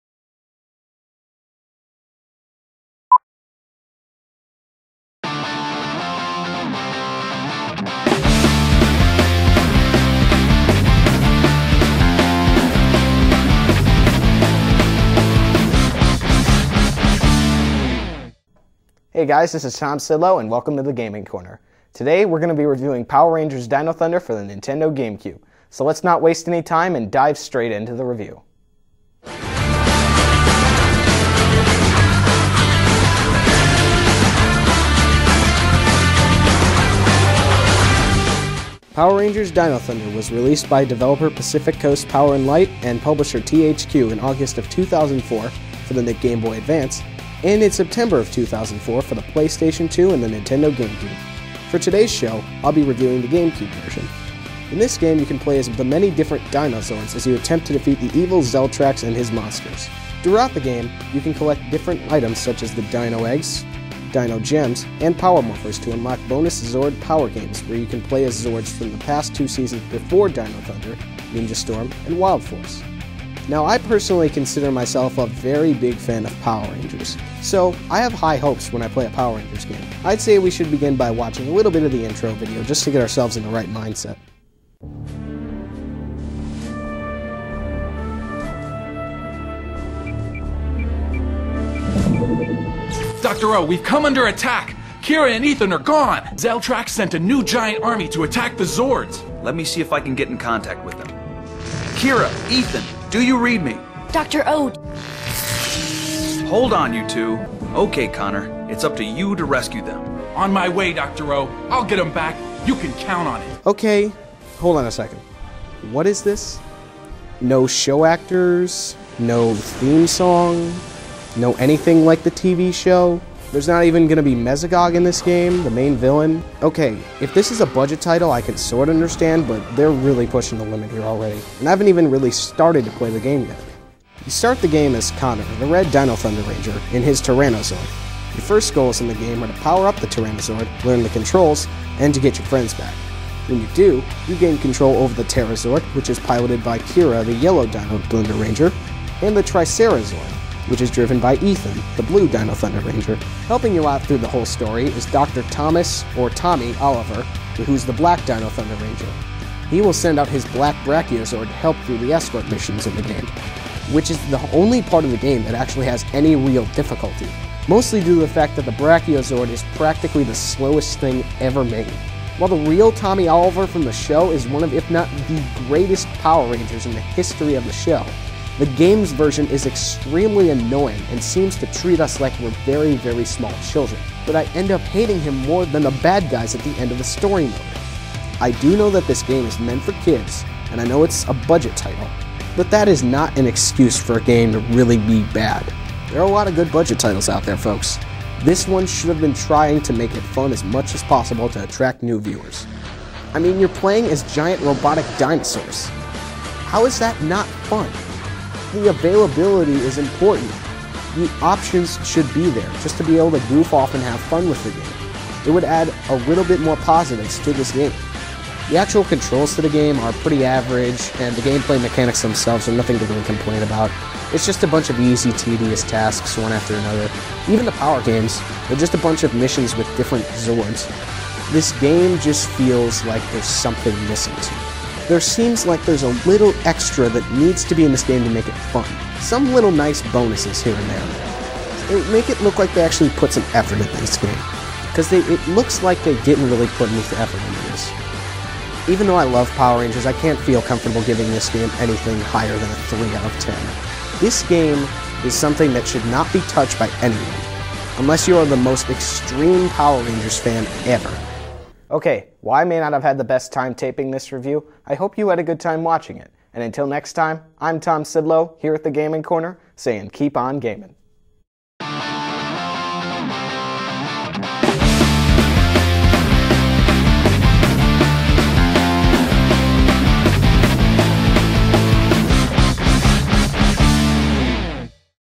The Gaming Corner: Power Rangers Dino Thunder
In the first episode of The Gaming Corner, "Mr. ihateslippy" Tom Szydlo reviews Power Rangers Dino Thunder. Is it as awesome as the first seasons of Power Rangers or does it fail like most of the Disney series do? Tune in to find out!

This was made for air on RBTV, Riverside-Brookfield High School's TV Network. If you like this review, there will be another soon!

Until next time, keep on gamin'!